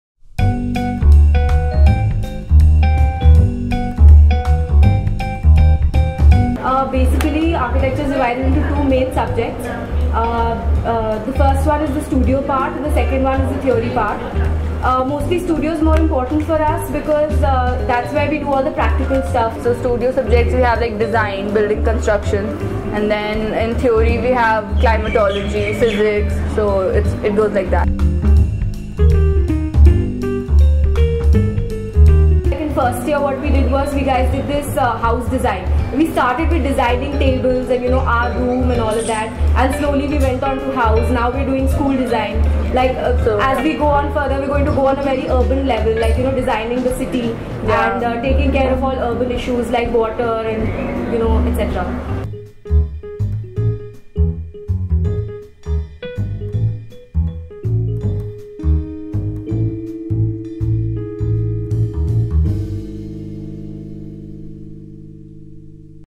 Architecture as a career (life in college) - One Step Up Education
Watch this video of Architecture students talking about the course